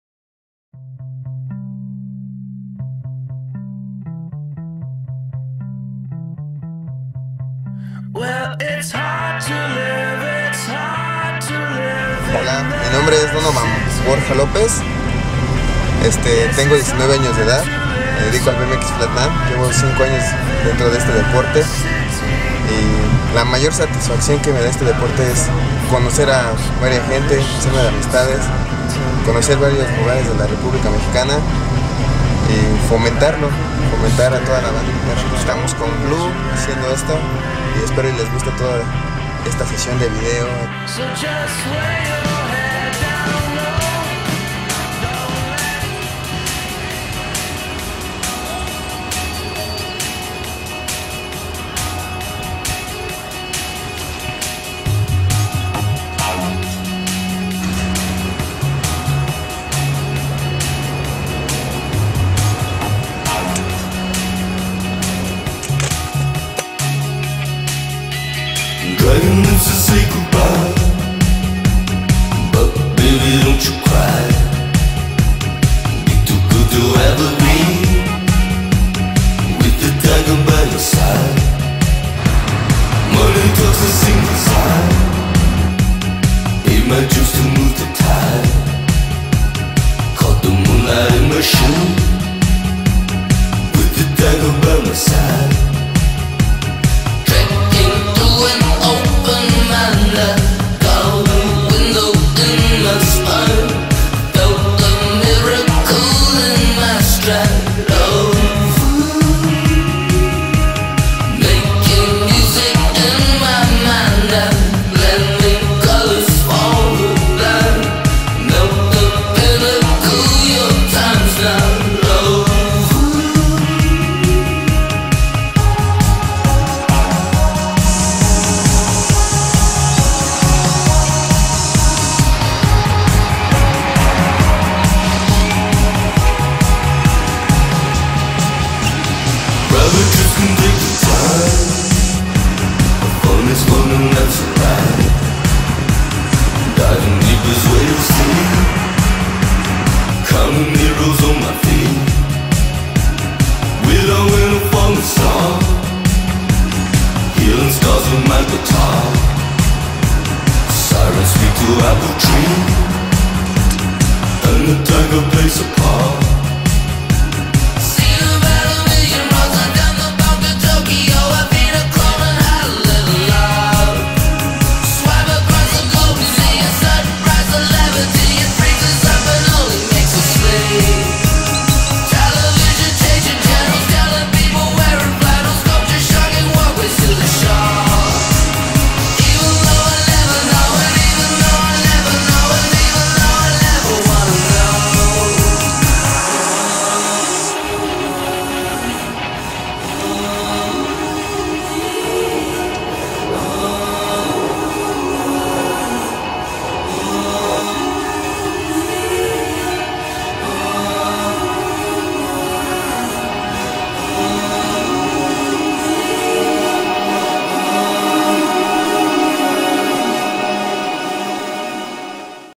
BMX FLATLAND MEXICO -DONOVAN BORJA
Donovan borja el "Donaz" como lo conocen es un amante del BMX, en su modalidad de Flatland, este es el primero de muchos videos junto con Blue fotografia